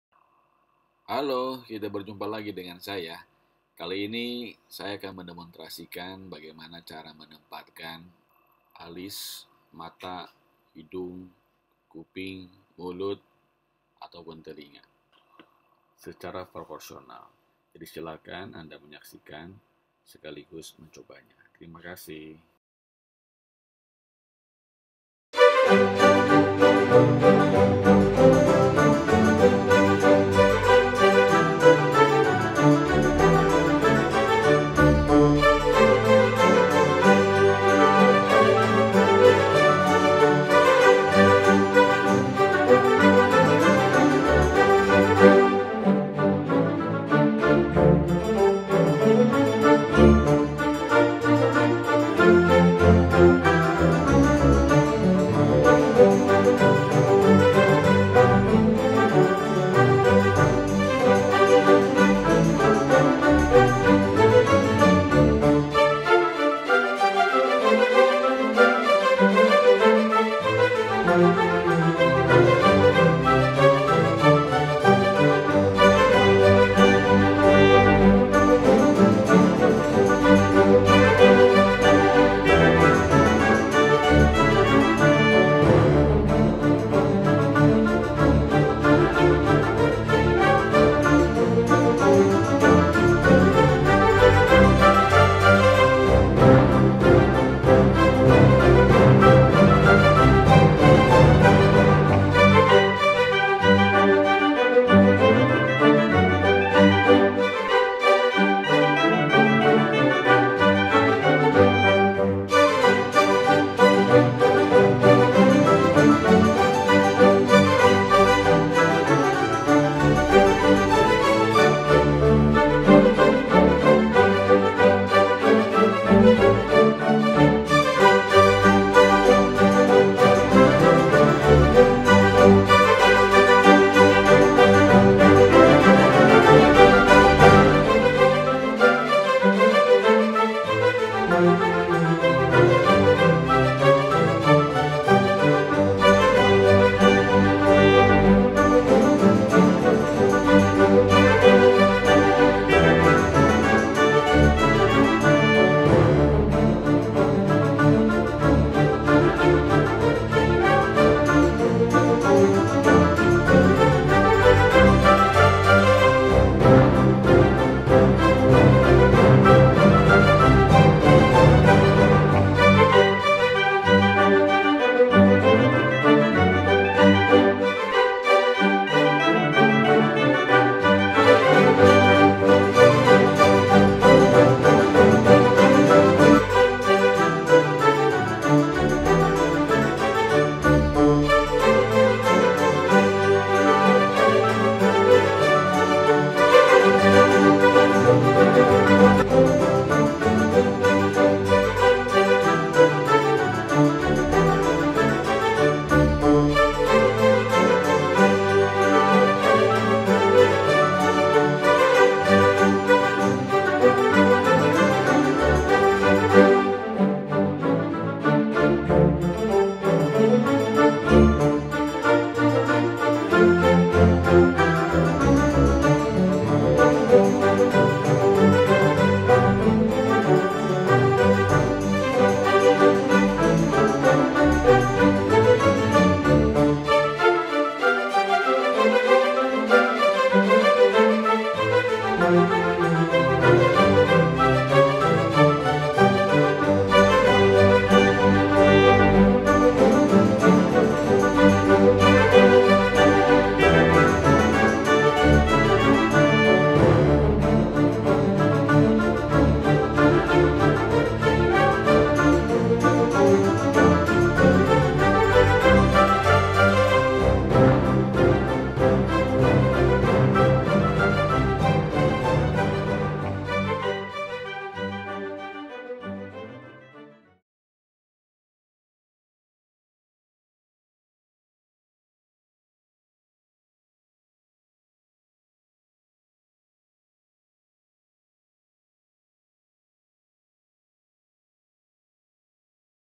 Teknik dan cara mudah menggambar wajah ala "Jack Hamm" #2
Video lanjutan setelah anda mencoba membuat struktur wajah di video sebelumnya, selamat menyaksikan. Bila ada pertanyaan silahkan menulis di kolom yang tersedia..Terimakasih